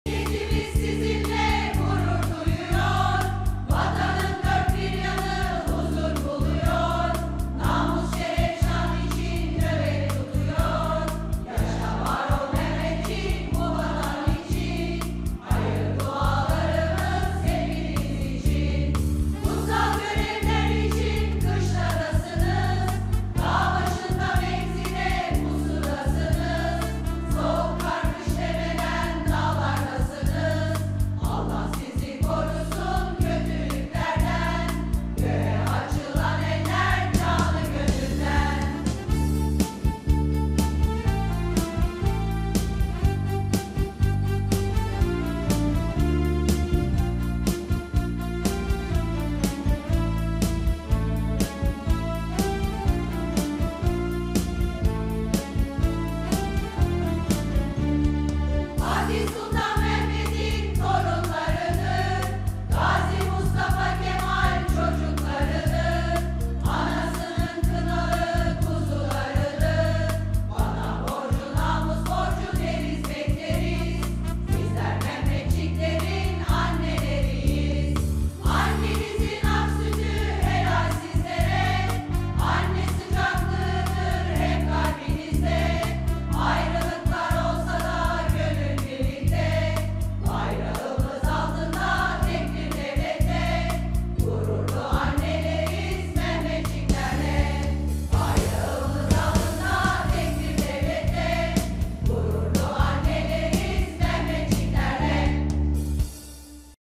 Kadın Korosu Mehmetçik için marş yazdı, söyledi
Bursa’da 105 kadının bir araya gelerek kurduğu Nilüfer Kadın Korosu, sınır ötesinde Barış Pınarı Harekatı’na katılan Mehmetçikler için daha önce yazdıkları bir marşı seslendirdi. Marşın söz ve müziğini yapan Koro Şefi Aysel Gürel seslendirdikleri marşı, ‘Anneleriniz daima arkanızda’ notuyla paylaştı.